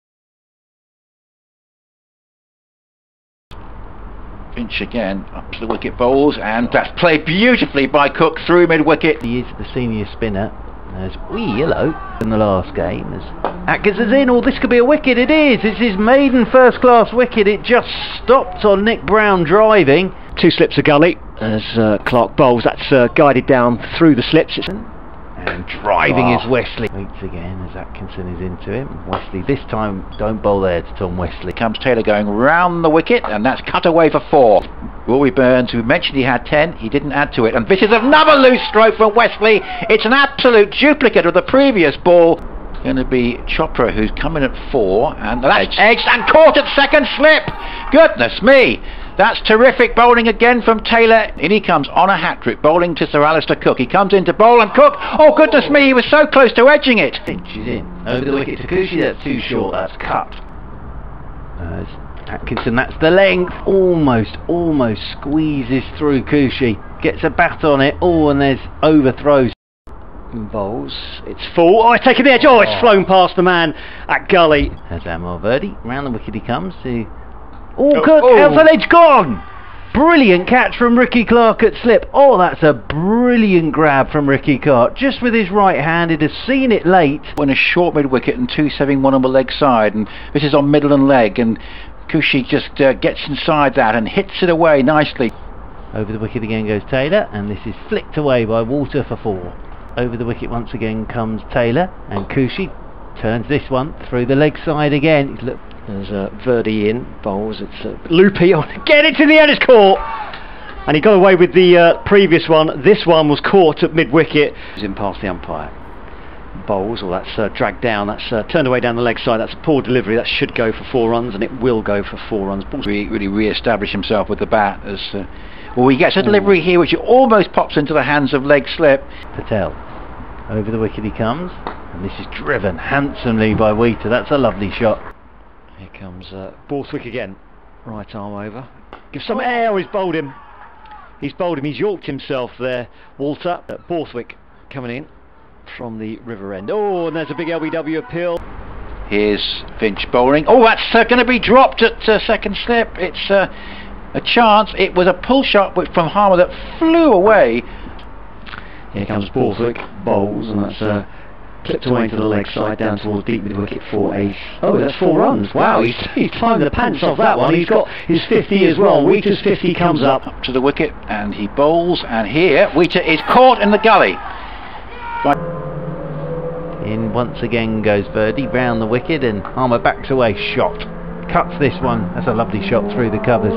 Atkinson & Taylor claim first Surrey wickets! | Essex v Surrey - Bob Willis Trophy Day 1 Highlights
Gus Atkinson & James Taylor enjoyed days to remember as they both took their first senior Surrey wickets on an otherwise highly competitive day of cricket at the Cloudfm County Ground in Chelmsford.